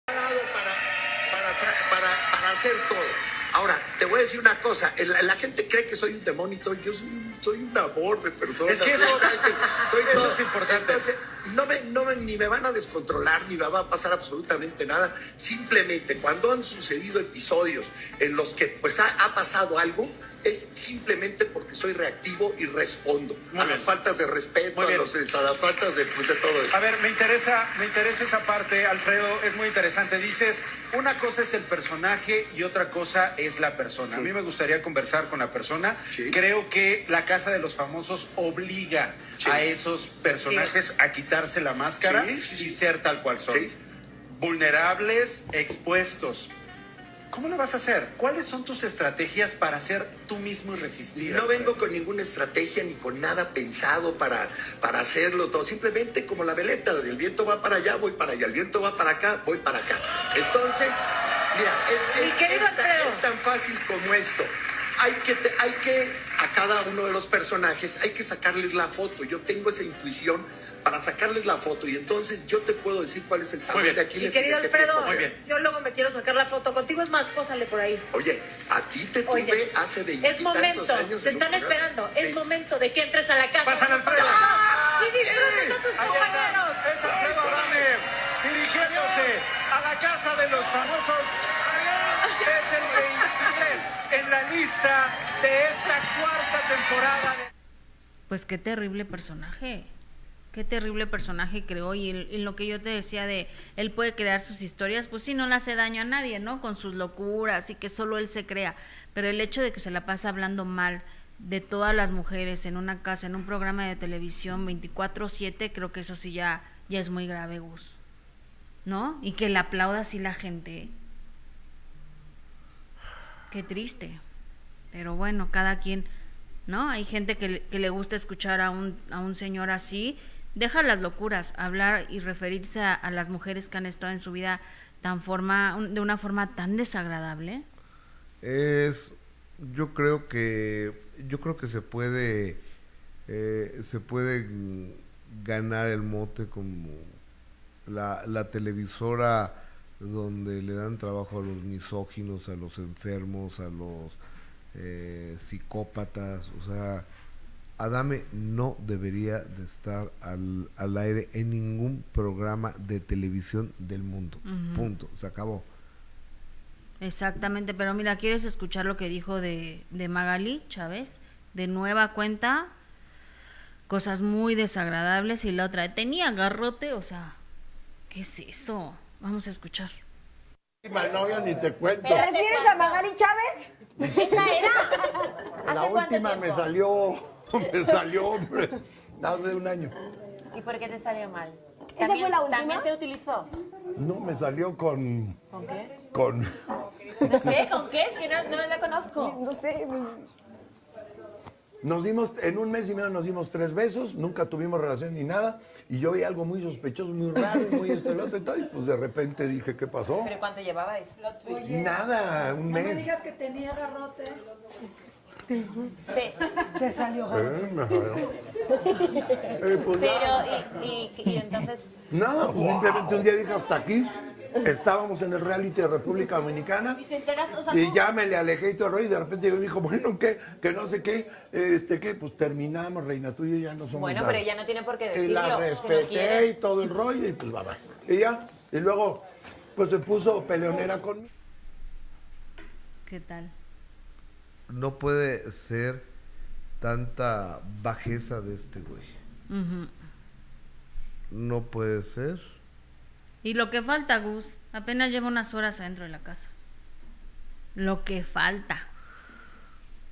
Adame le tira con todo a Magaly Chávez “era HOMBRE”
“Era HOMBRE” AlfredoAdame le tira con todo a MagalyChávez